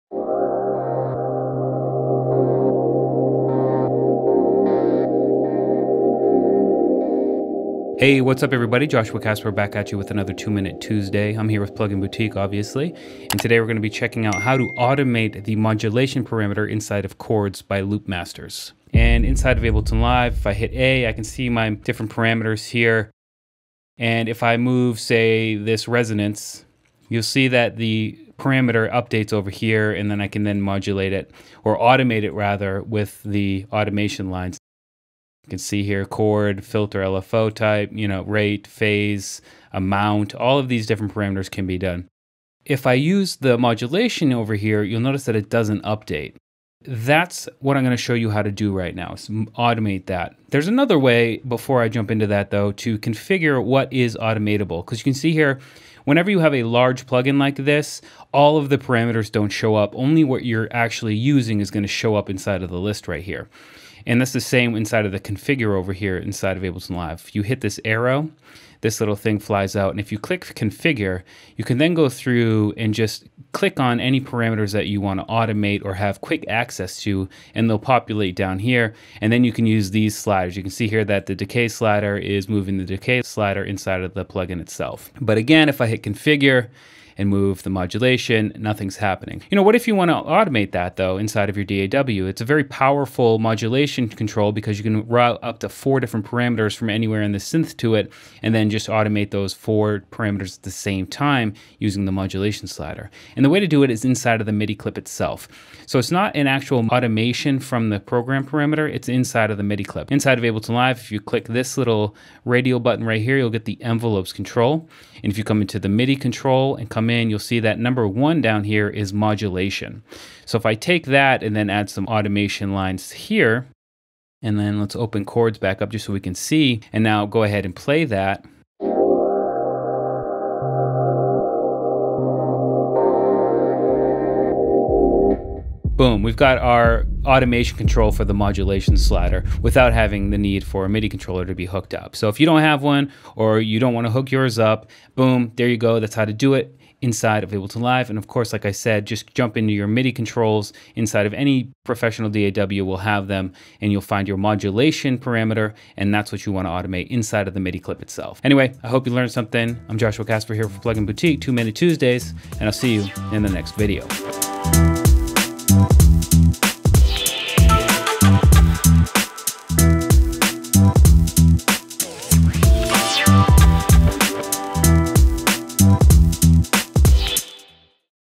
TMT | Khords by Loopmasters | Automate the Modulation Slider Tutorial
In this video tutorial, for our Two Minute Tuesday series, Joshua Casper explains how you can automate the modulation slider in Khords.

While any parameter in Khords can be automated via the usual automation methods in any professional DAW (Ableton Live, Cubase, Logic, Reason, Bitwig, Etc.) the modulation slider is automatically connected to / looking for a MIDI controller.

If you don't have one, or if you don't want to plug yours in or if you just don't want to use it, you will need to edit the Modulation MIDI parameter inside any MIDI clip itself.